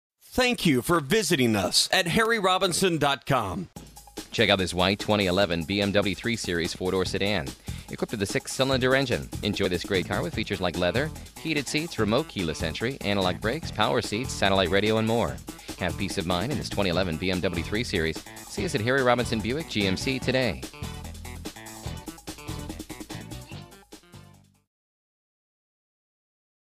BMW Series, Harry Robinson Buick GMC- Fort Smith, AR 72908
BMW Series, Harry Robinson Buick GMC ready for the very best? let Harry Robinson Buick GMC show you what luxury really means. imagine driving this Alpine White 2011 BMW 3 Series, equipped with a 6 Cyl. engine and an automatic transmission with 35,156 miles. enjoy an impressive 28 miles to the gallon on this luxury car with features like satellite radio, cruise control, gps navigation system, automatic climate control, dual climate control, leather upholstery, power sunroof, anti-lock braking system, backup sensors, and much more. Enjoy the drive and have peace of mind in this 2011 BMW 3 Series. see us at Harry Robinson Buick GMC today!

miles- 35,156

vin- WBAPM5G50BNM68623

6000 S. 36th Street